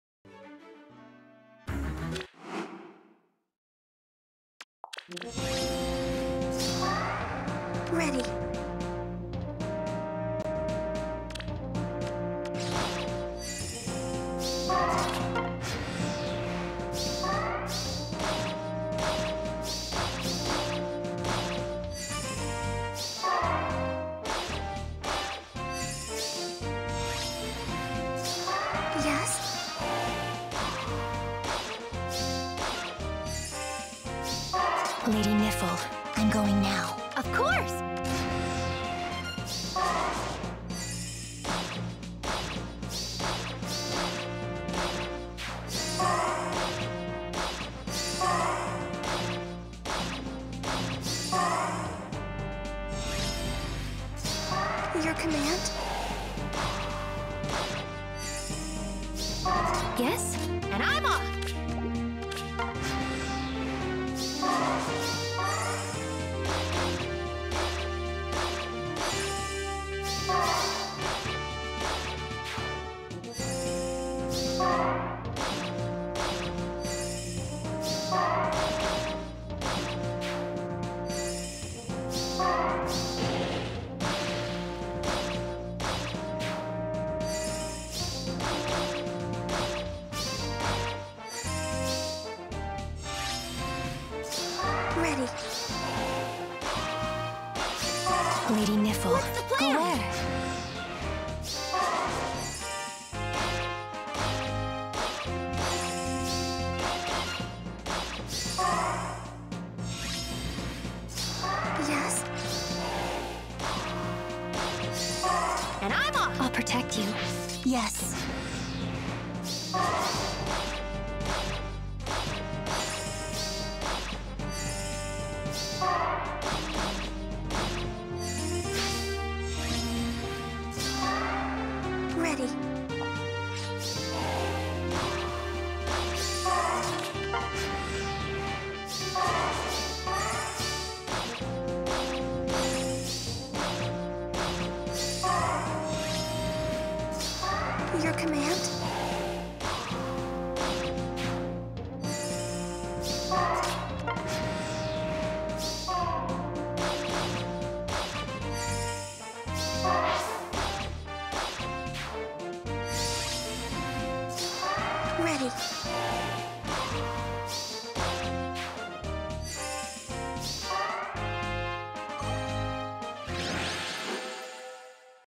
Katarina True Solo - Abyssal Leif LHB
Katarina | +atk +def | 10 merges | 20 dragonflowers | Summoner Support S

⚔️Rauðrvulture+ (def)
🔥Blazing Wind
🅐Close Foil
🅑Lull Atk/Res 3
🅒Atk Smoke 4
🆂Mystic Boost 3